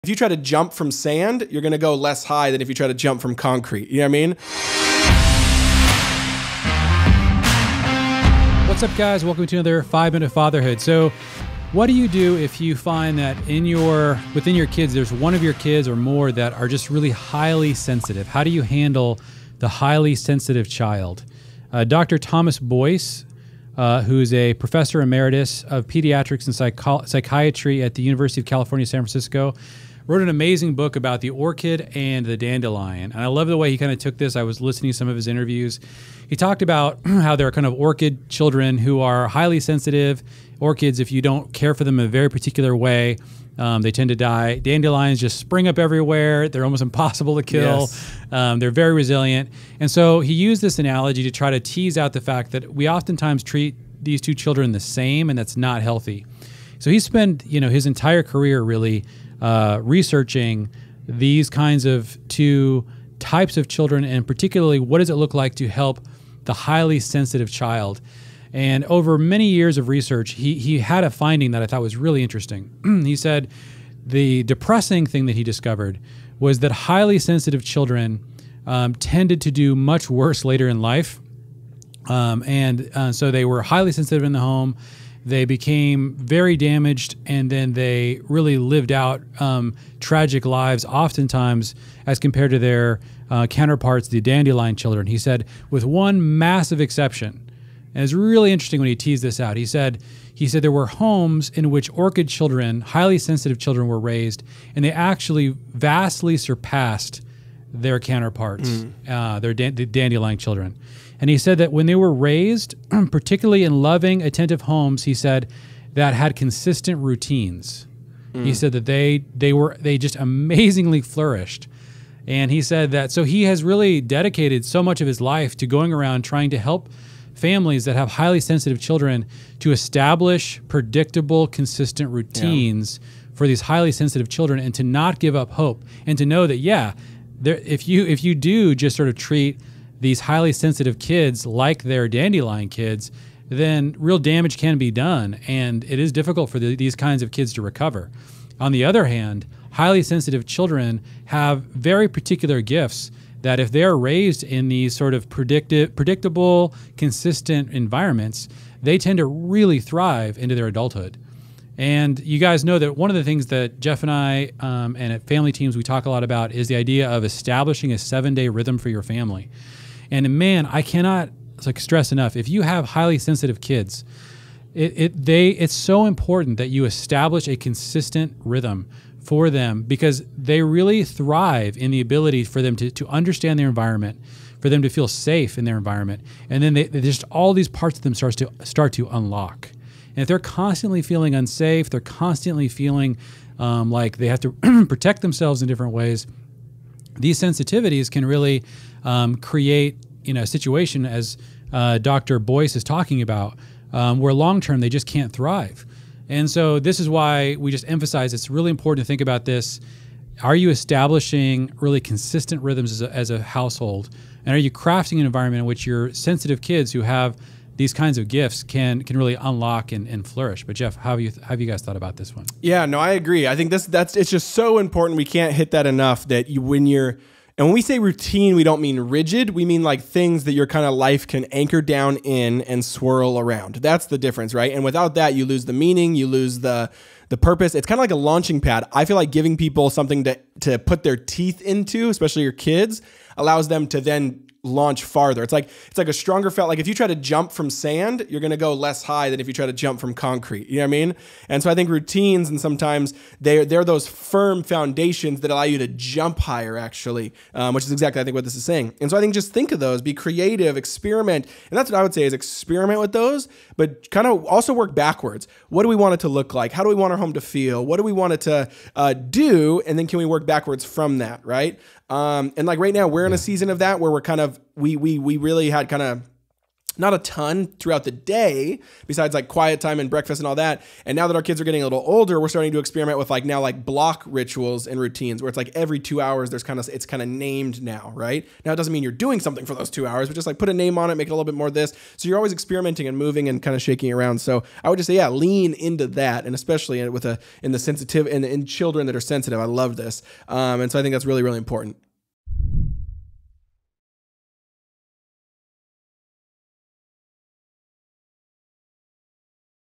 How do you Handle a Highly Sensitive Child? | Ep. 225
Jeff Bethke and Jeremy Pryor talk about what to do with a sensitive child.